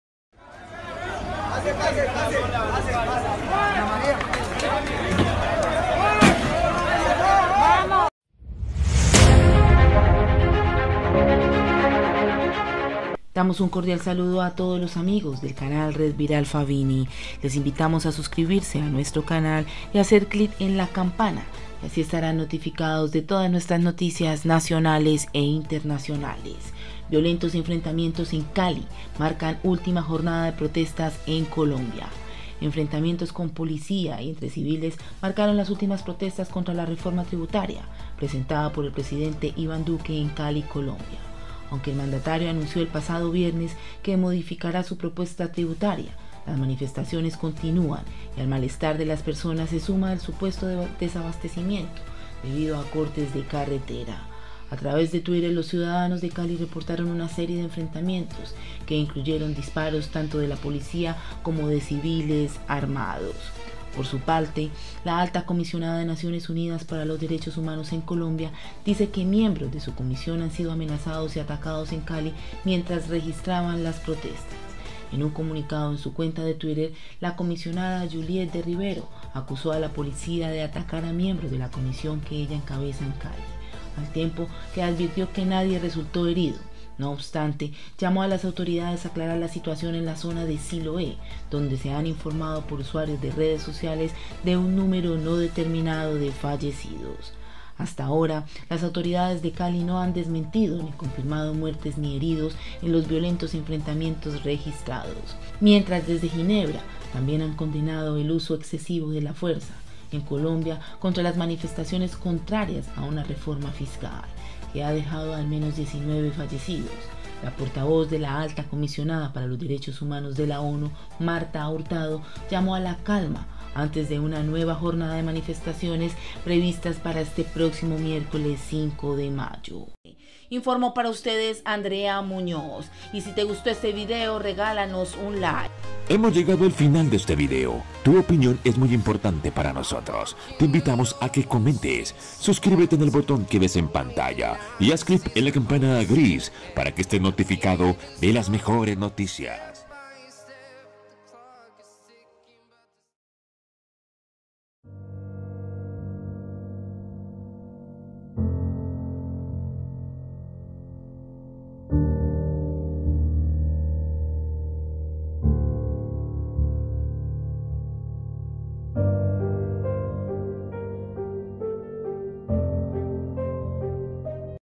ATENCIÓN ¡ ÚLTIMA HORA ! ESTÁ ES LA REALIDAD QUE SE VIVE CONTINUÁN LAS PRO-TESTAS EN CALI- COLOMBIA
¡ ATENCIÓN ! HACE UNAS HORAS ENFREN-TAMIENTOS EN CALI MARCAN ULTIMA JORNADA DE PRO-TESTAS EN COLOMBIA